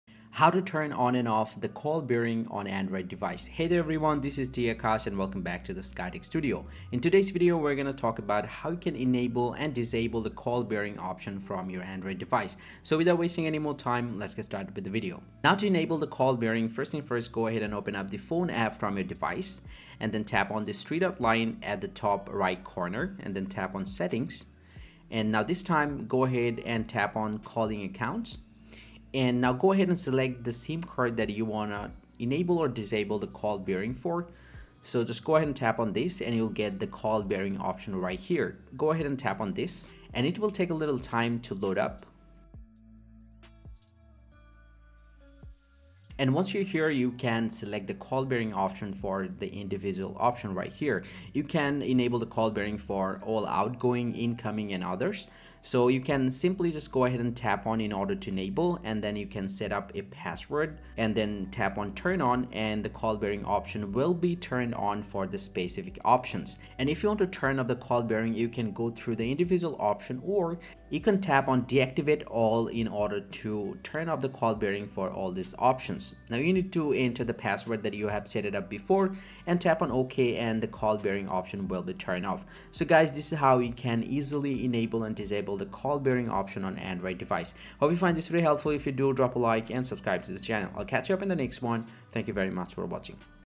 How To Turn On/Turn off Call Barring on Any Android | Call Barring Settings For Android
How to turn off Call Barring on Android?
How to turn on Call Barring on Android?

Do you want to enable and disable call barring on Android? If you want to turn off or turn on call barring for Android and want to know about call barring settings or incoming or Outgoing call barring then you are in the right place. In today's tutorial, we are going to see what is call barring? and How to Setup Call Barring Incoming and Outgoing?

let's explore!

00:01- Video Starting
00:15- Main Process
01:22- Process Ending